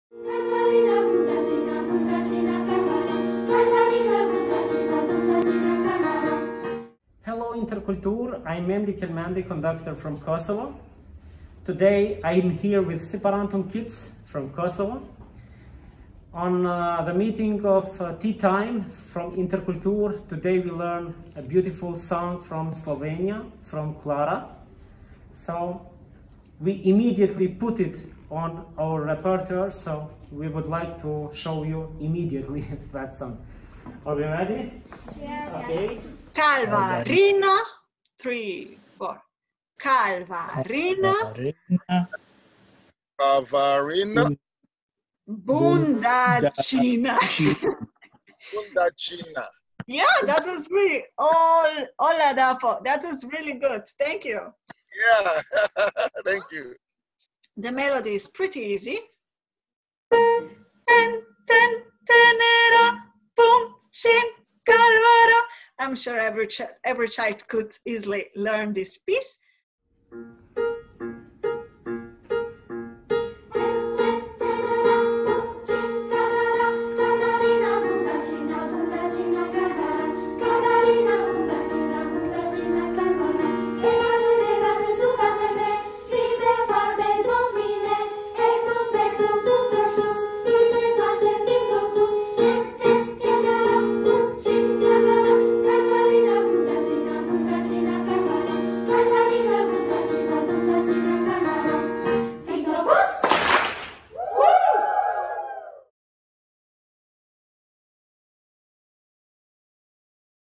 Singing together brings nations together: From a virtual space to the rehearsal room
Since spring 2020 INTERKULTUR invites choir singers, conductor and everybody interested regularly to "INTERKULTUR Tea Time", an informal online meeting for conversations on different topics.

Here you can see what happened, when Memli Kelmendi, conductor from Kosovo, learned about music from Slovenia during INTERKULTURTeaTime and a few hours later had choir practice with his choir "Kori i fëmijëve Siparantum Kids"